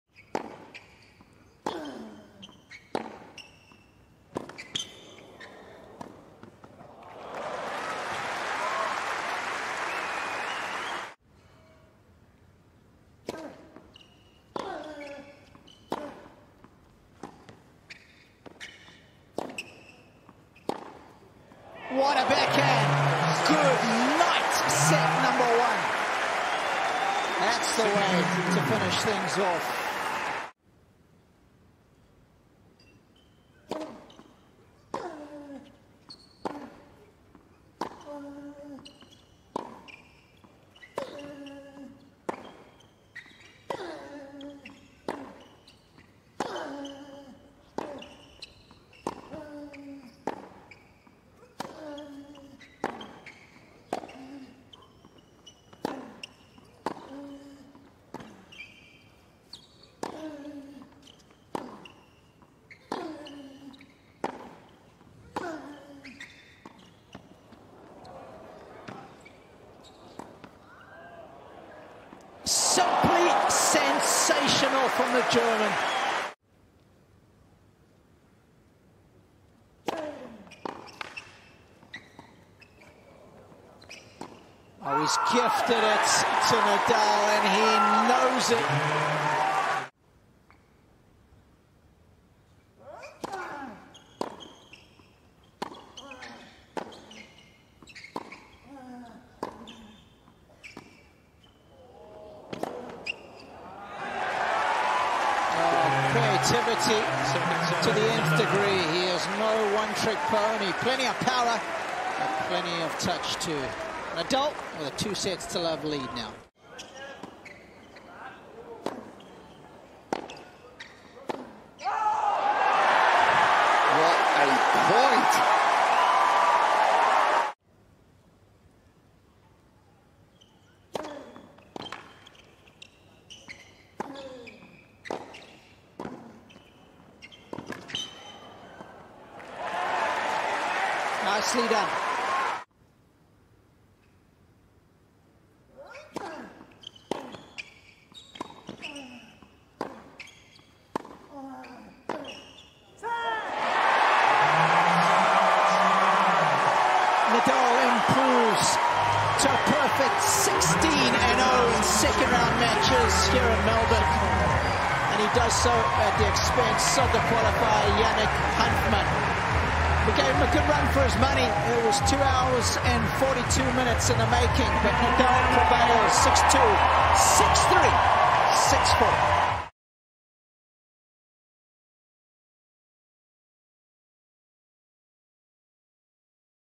Yannick Hanfmann v Rafael Nadal Highlights (2R) | Australian Open 2022
Yannick Hanfmann takes on Rafael Nadal in the second round of the Australian Open 2022.